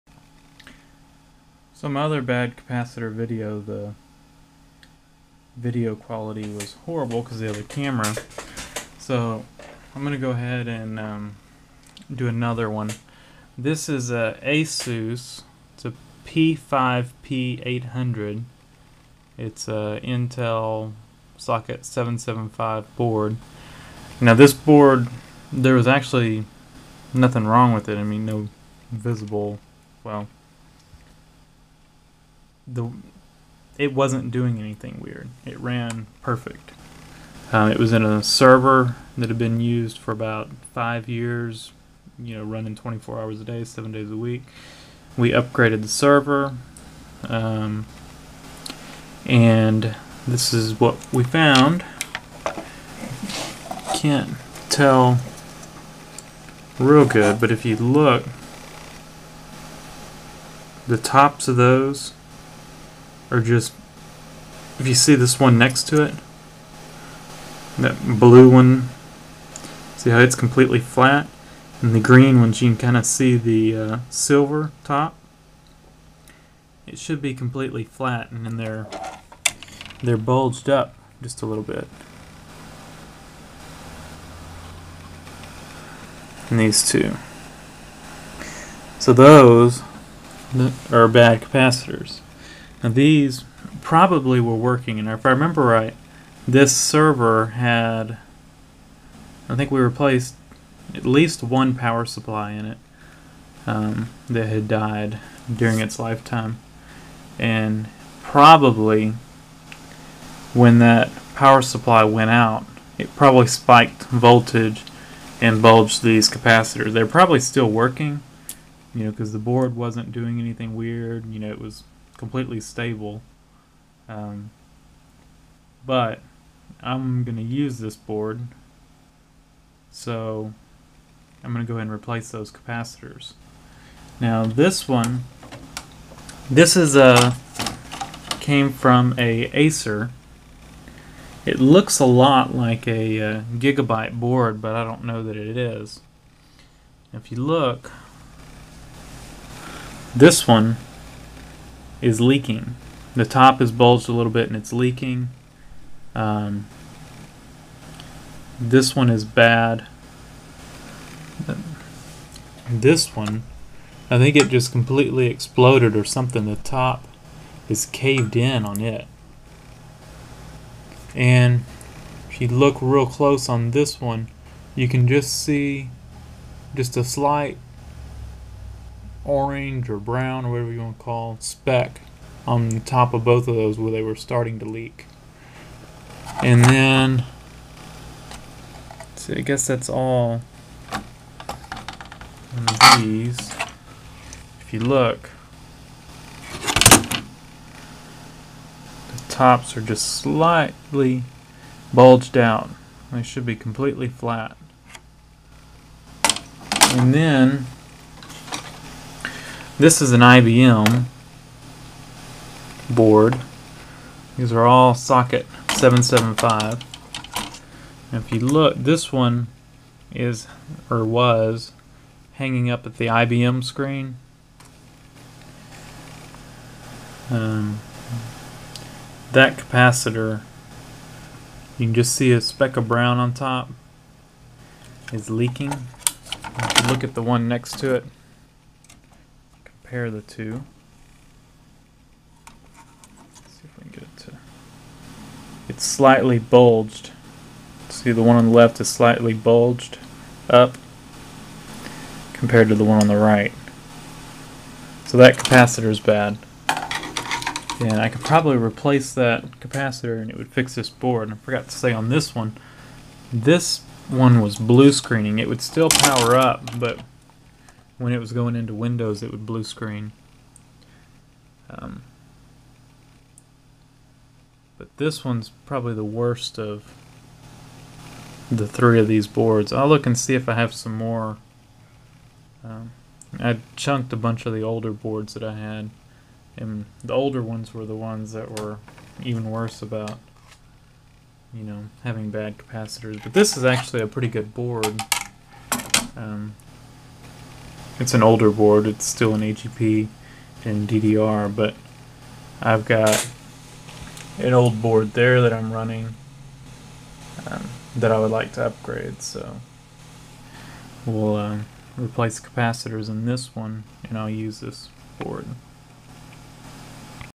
Identifying bad capacitors part 1
I previously made a video about bad capacitors, but the quality of my old camera was horrible. This is a new video with my new camera, showing how to identify bad capacitors on a motherboard.